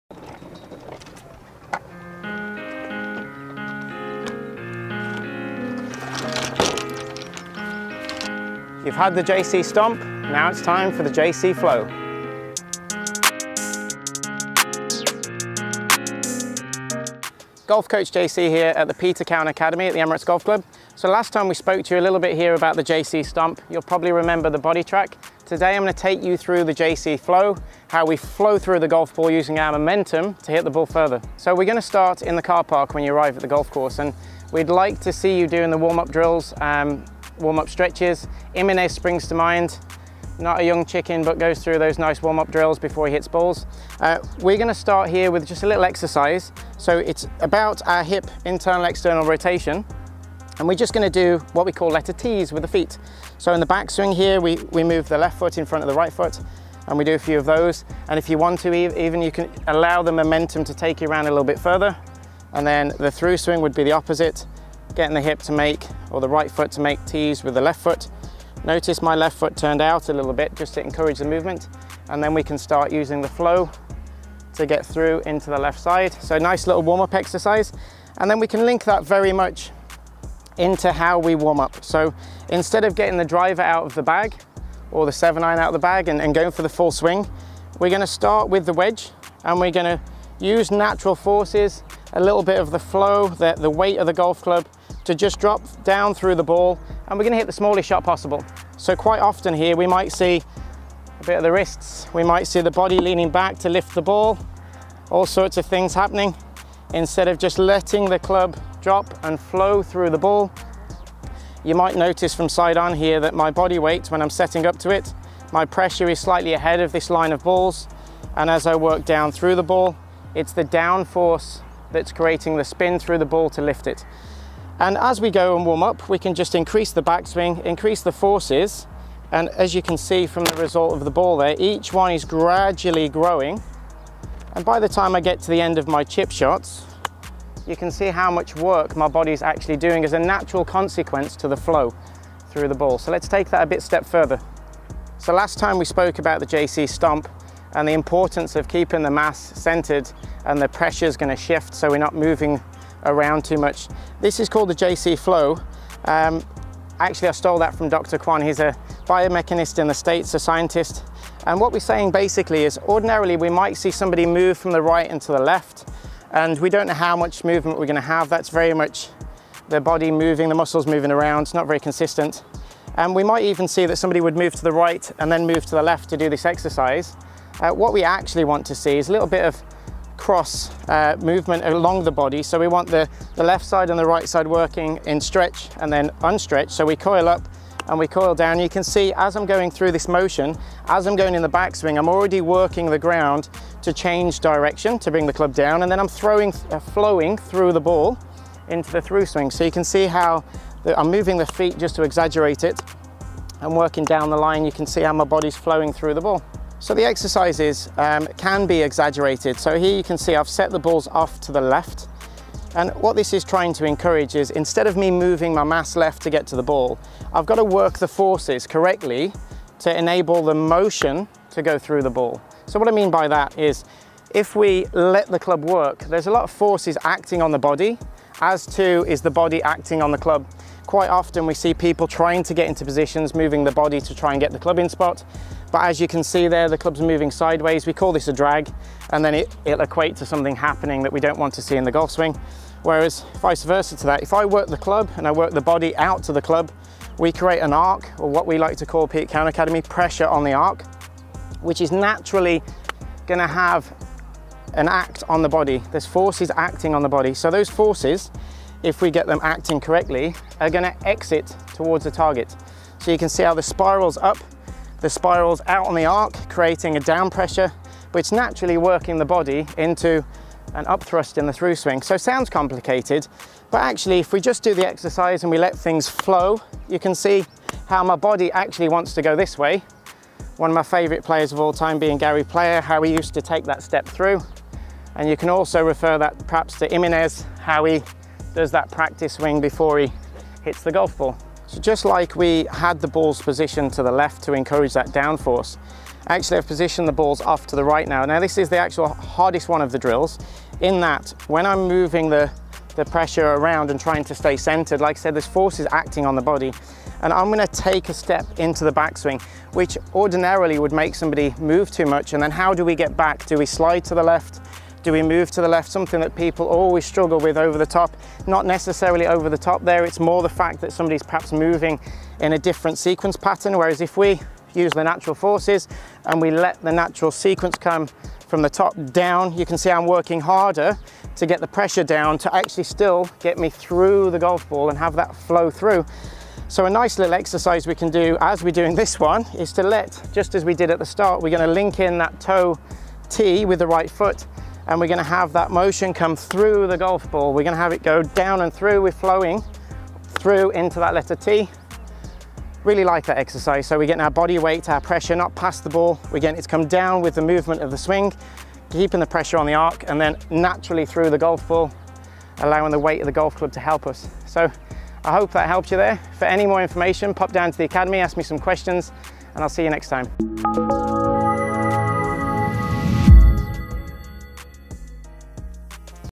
Bomb the Ball with your natural swing. The JC Flow
One of my favourite drills the JC Flow, you don’t need all of the balls demonstrated 5 balls in a row will do the trick. Practice positioning the balls off in diagonals to increase the correct pressure movement in the ground which in turn will lead to the correct centre of mass movement.